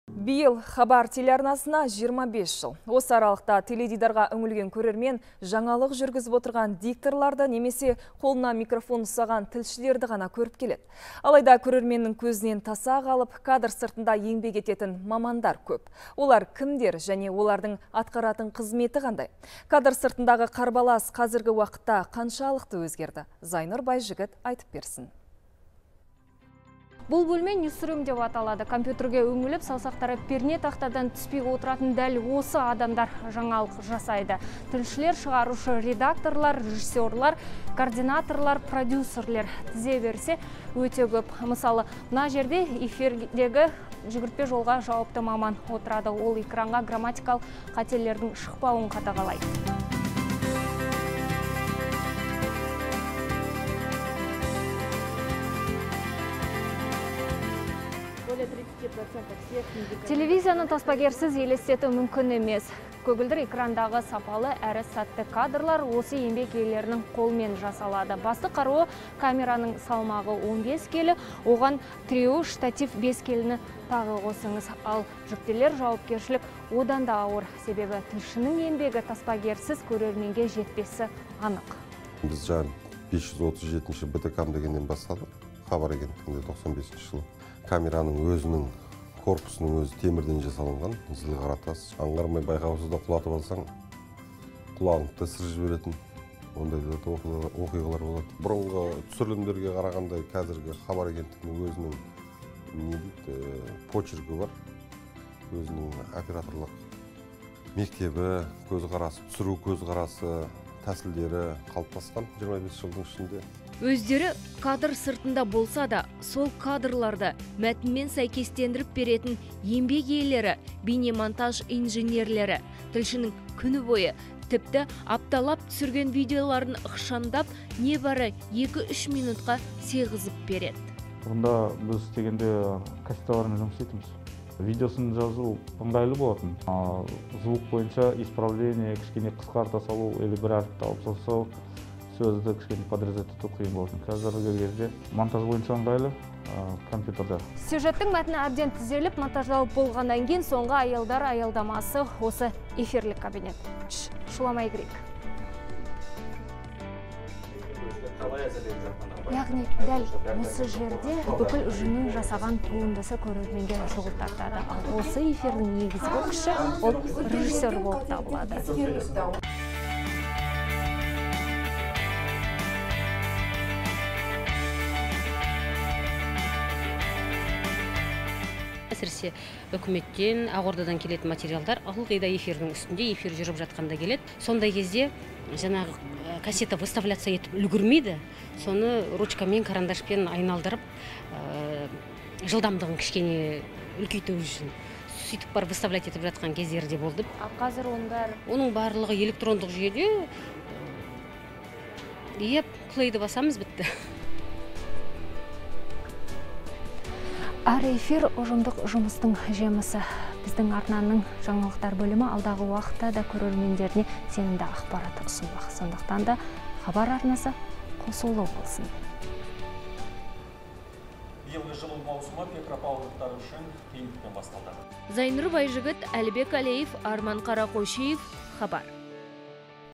«Хабарға» 25 жыл: Кадр сыртындағы қарбалас. Бұрын және қазір
Биыл «Хабар» телеарнасына – 25 жыл.

Осы аралықта теледидарға үңілген көрермен жаңалық жүргізіп отырған дикторларды немесе қолына микрофон ұстаған тілшілерді ғана көріп келеді. Алайда көрерменнің көзінен таса қалып, кадр сыртында еңбек ететін мамандар көп. Олар кімдер? Және олардың атқаратын қызметі қандай? Кадр сыртындағы қарбалас, қазіргі уақытта қаншалықты өзгерді?

Зайнұр Байжігіт, тілші:

– Бұл бөлме ньюс-рум деп аталады. Компьютерге үңіліп, саусақтары пернақтадан түспей отыратын дәл осы адамдар жаңалық жасайды. Тілшілер, шығарушы редакторлар, халықаралық жаңалықтар бөлімінің қызметкерлері, продюсерлер, координаторлар. Тізе берсе, өте көп. Бұл жерде экрандағы жүгіртпе жолға жауапты маман отырады. Ол грамматикалық қателердің эфирге шықпауын қадағалайды.